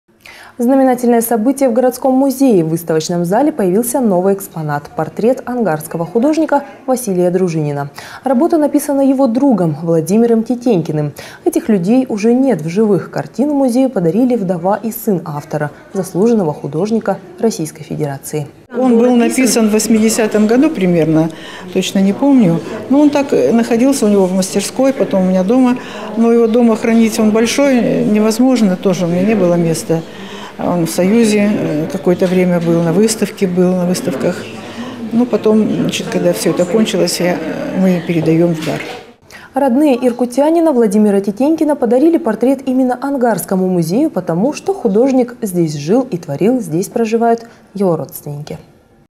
Городской Музей пополнился портретом Василия Дружинина. 29-06-2012
Работа написана его другом, Владимиром Тетенькиным. Этих людей уже нет в живых. Картину Музею подарили вдова и сын автора, заслуженного художника Российской Федерации.

 Эльвира Элиас, вдова художника Владимира Тетенькина: - Написан он в 1980-ом году, точно уже не помню когда. Он находился сначала в его мастерской, потом у меня дома, но дома хранить его невозможно - он очень большой, а у меня не было места. В союзе художников какое-то время был, на выставках. Потом, когда все это кончилось, мы передаем это в дар.

 Родные иркутянина Владимира Тетенькина подарили портрет именно Ангарскому музею потому, что художник здесь жил и творил, здесь проживают его родственники.